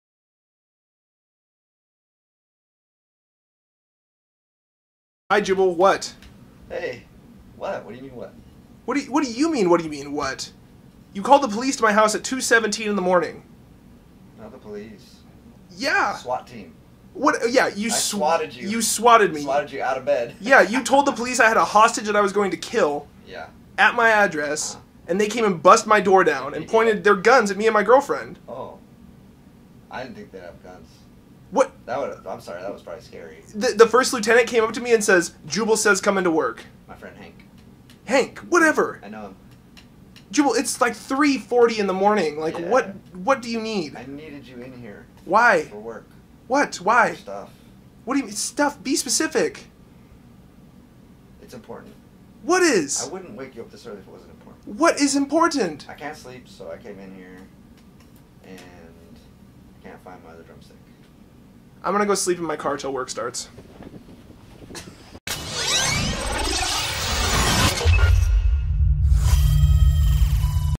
:60 with Jubal - Early Mornings
When it's 2AM and you can't sleep and you just want a friend, but you know that everyone is sleeping so you just lay there awake and alone...yeah, Jubal doesn't play by those rules.